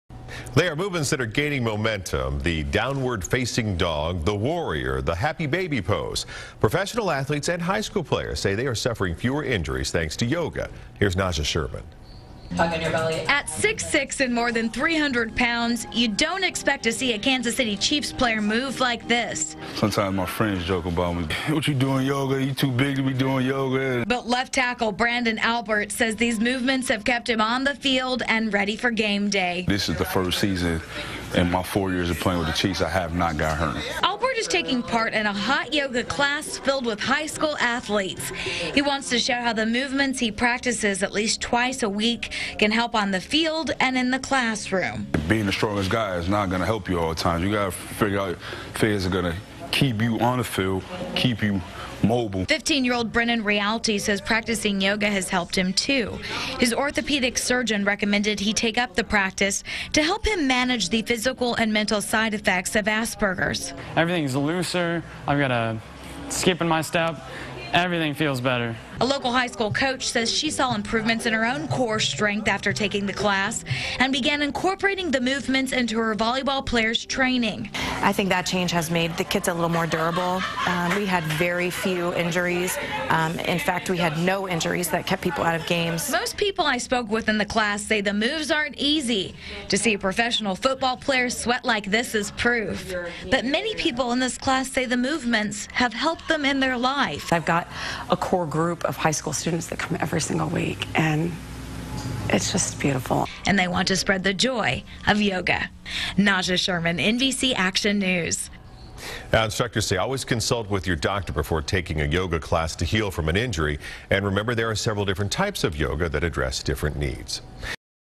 Chiefs player switches from tackles to yoga
A left tackle in the NFL is thought to be big and bruising, but the Chiefs Branden Albert says turning to yoga is improving his on-field play.